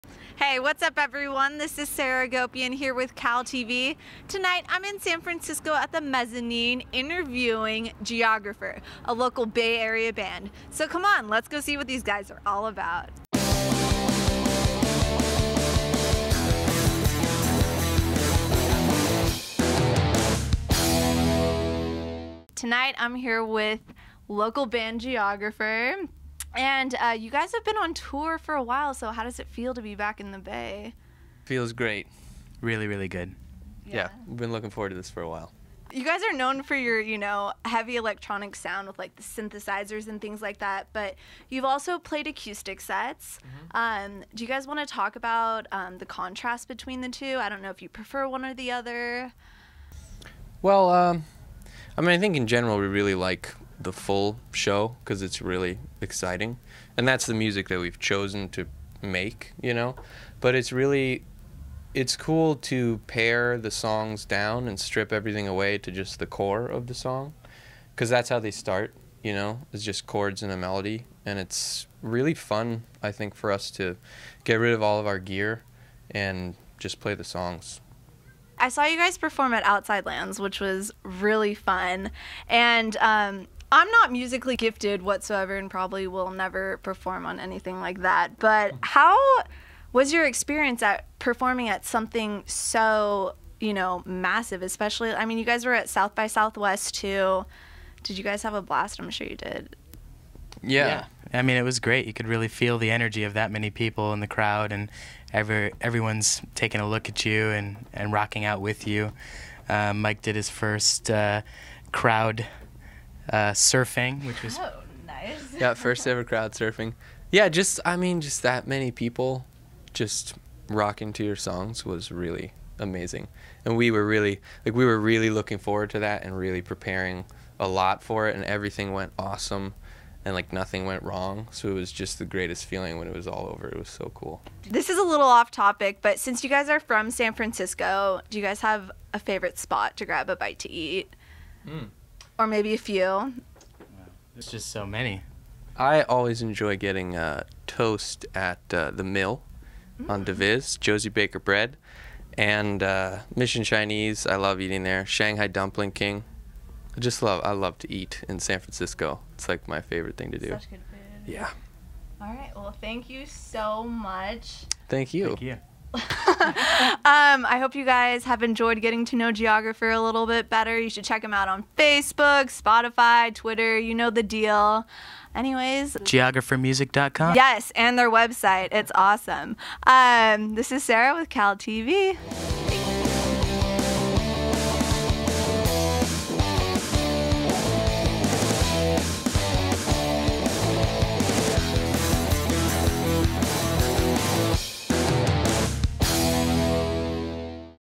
CalTV E: Chatting it up with Geographer
Join Sarah Agopian from CalTV Entertainment as she talks with local Bay Area band Geographer.

Follow us @CaltvE.

Credits
Reported by: Sarah Agopian
Filmed by: Lisa Rosen
Edited by: Jason Piemnoppakao